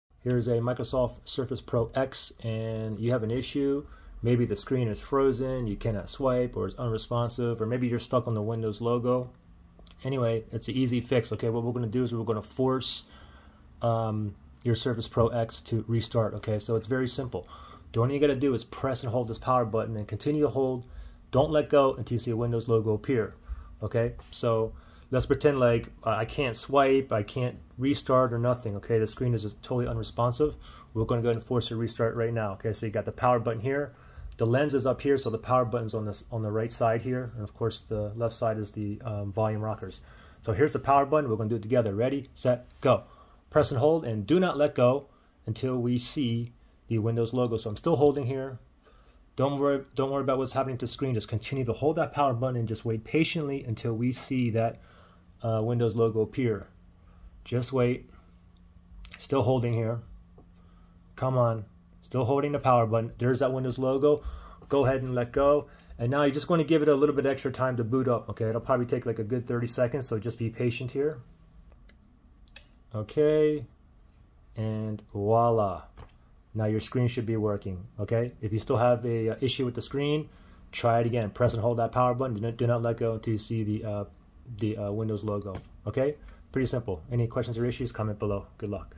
Microsoft Surface Pro X: Stuck, Frozen or Unresponsive Screen? Fixed!
I show you how to fix a frozen or unresponsive screen or stuck on Windows logo on the  Microsoft Surface Pro X. Hope this helps.

Microsoft Surface Pro X – 13" Touch-Screen